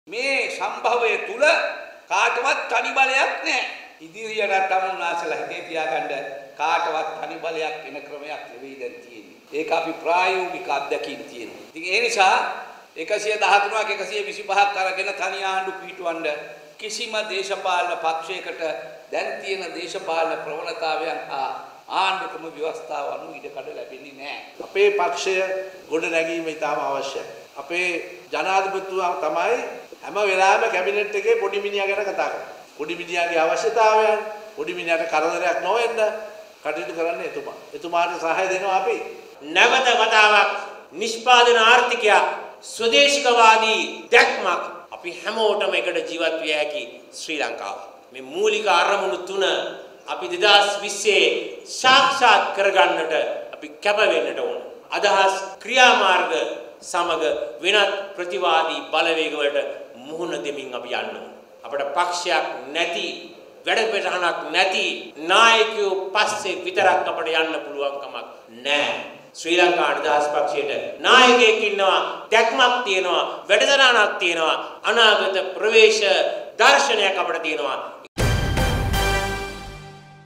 No journey for SLFP with leaders with no party or vision - SLFP General Secretary
පක්ෂයක් - දැක්මක් නැති නායකයන් පසුපස ශ්‍රී ලංනිපයට ගමනක් නෑ